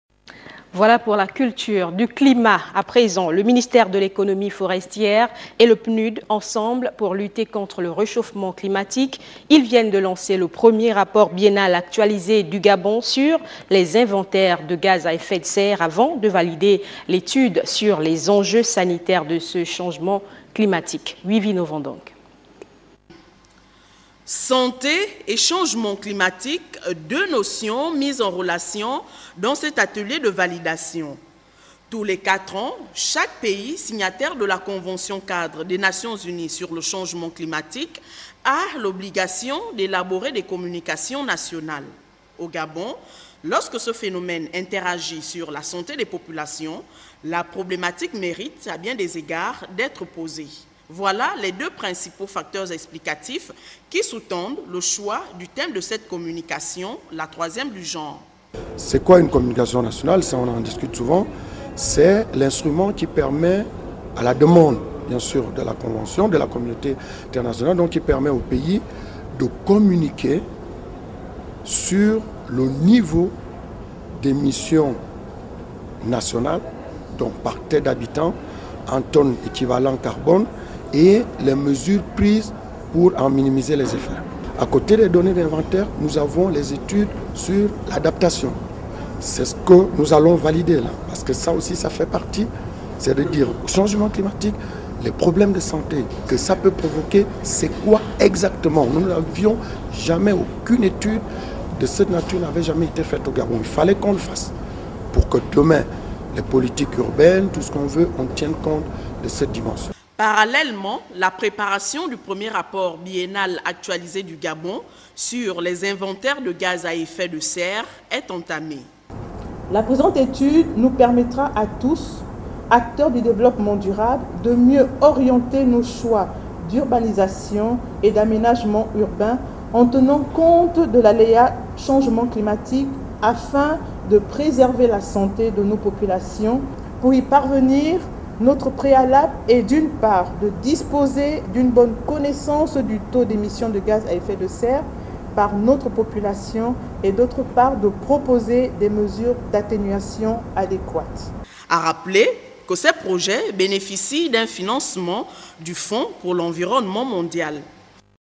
RTG / Lancement du 1er rapport biennal actualisé sur les inventaires des gaz à effet de serre
Lancement du premier rapport biennal actualisé du Gabon sur les inventaires des gaz à effet de serre